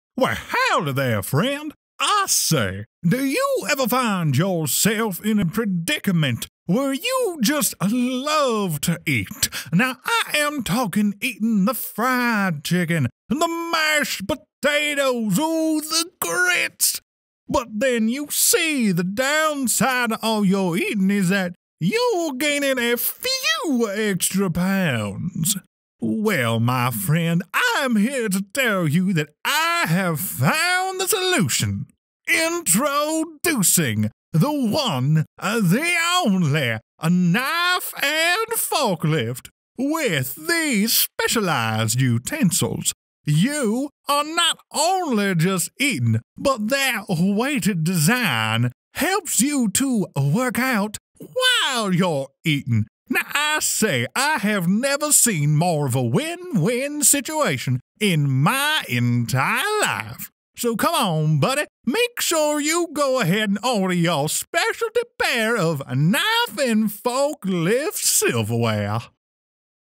Knife and Fork Lift - Voiceover Trial 5
Another product for you all. This one was apprently featured on the Today show. Don't know what someone would need with weighted silverware, but to each his own I suppose.